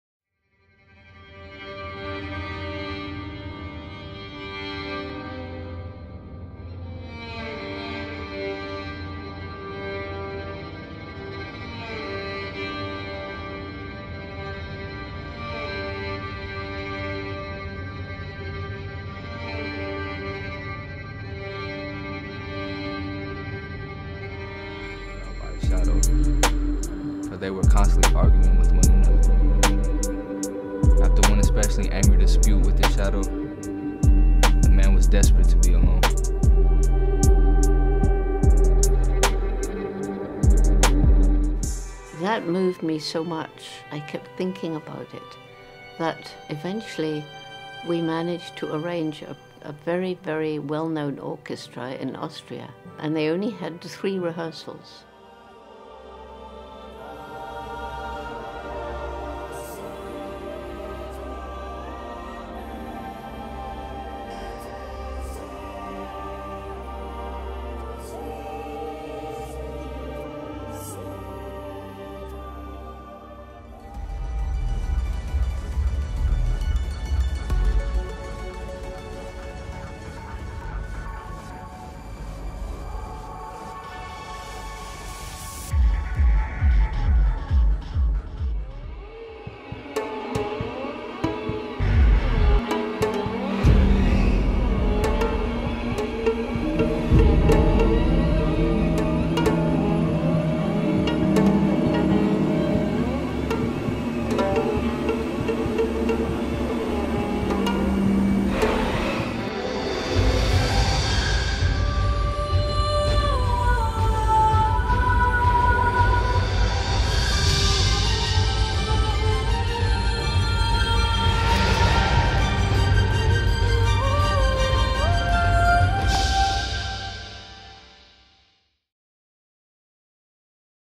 2021 Film Composer Demo Reel | Adele Etheridge Woodson
New year, new me...in June? Happy to share my newest demo reel with some scores from the past year that I am extra proud of.